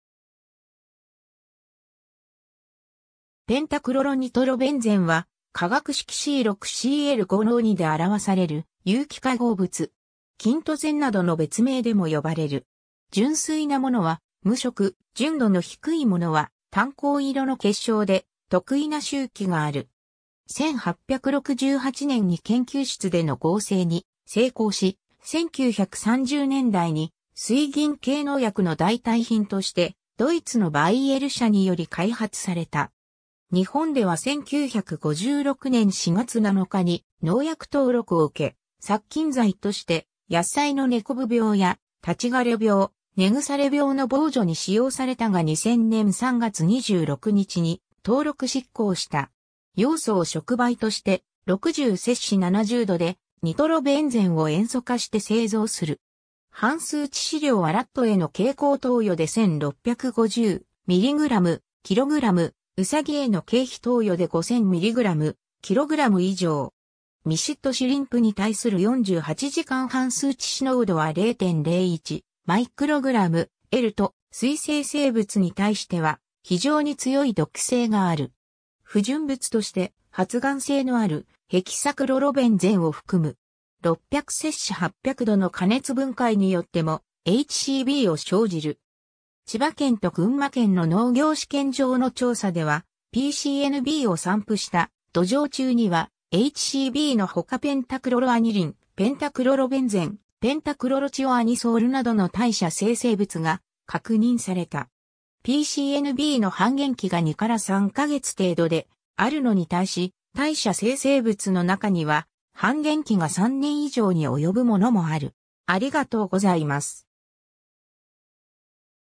ペンタクロロニトロベンゼン
ペンタクロロニトロベンゼン（英: pentachloronitrobenzene、略称PCNB）は化学式C6Cl5NO2で表される有機化合物。
キントゼン（quintozene）などの別名でも呼ばれる。
純粋なものは無色、純度の低いものは淡黄色の結晶で、特異な臭気がある。
 1868年に研究室での合成に成功し、1930年代に水銀系農薬の代替品としてドイツのバイエル社により開発された。
日本では1956年4月7日に農薬登録を受け、殺菌剤として野菜の根こぶ病や立枯れ病、根腐れ病の防除に使用されたが2000年3月26日に登録失効した。
 ヨウ素を触媒として、60-70℃でニトロベンゼンを塩素化して製造する。
 半数致死量（LD50）はラットへの経口投与で1,650mg/kg、ウサギへの経皮投与で5,000mg/kg以上。
ミシッドシュリンプに対する48時間半数致死濃度（LC50）は0.01μg/Lと水生生物に対しては非常に強い毒性がある。
不純物として、発癌性のあるヘキサクロロベンゼン（HCB）を含む。
600~800℃の加熱分解によってもHCBを生じる。
千葉県と群馬県の農業試験場の調査では、PCNBを散布した土壌中にはHCBのほかペンタクロロアニリン、ペンタクロロベンゼン、ペンタクロロチオアニソールなどの代謝生成物が確認された。
PCNBの半減期が2~3か月程度であるのに対し、代謝生成物の中には半減期が3年以上に及ぶものもある。